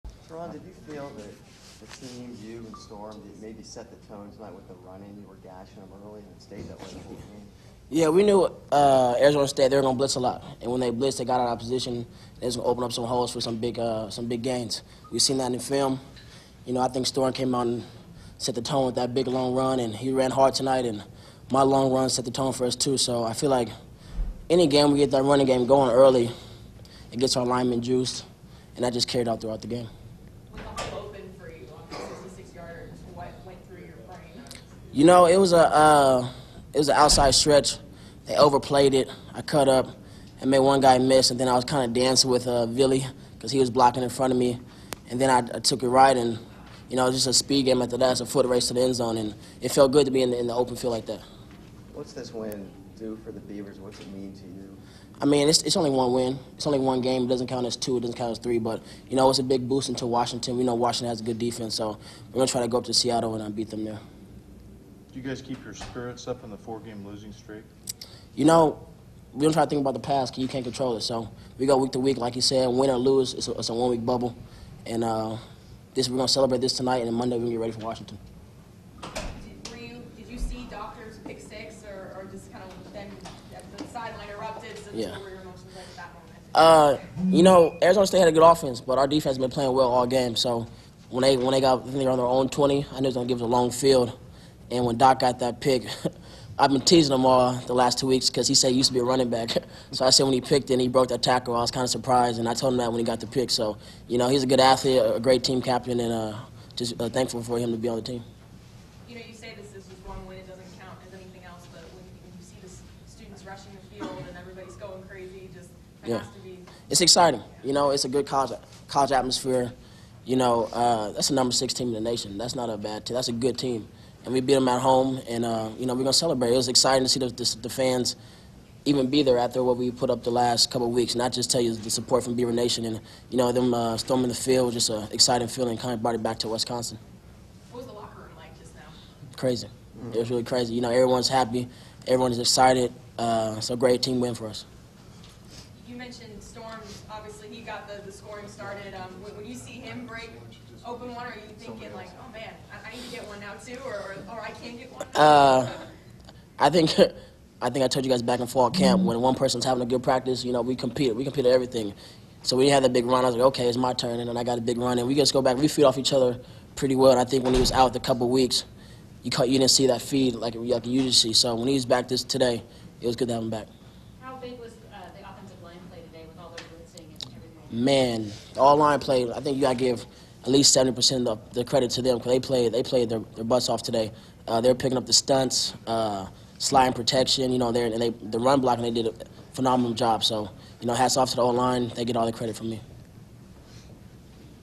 Terron Ward after Oregon State's 35-27 win over #6 Arizona St
Oregon State running back Terron Ward speaks with the media after the Beavers upset #6 Arizona State 35-27 at Reser Stadium on November 15, 2014. Ward had 19 carries for 148 yards and a touchdown